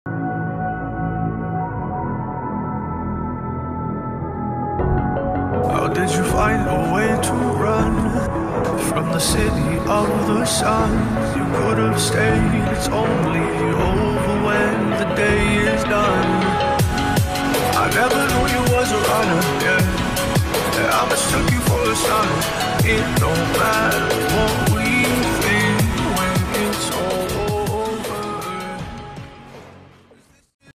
150KG x 9 Squats!! (VERY EASY)
Smolov workout, this was set 1 of 150kg x 9, did 4 sets in total, first set felt very easy 👍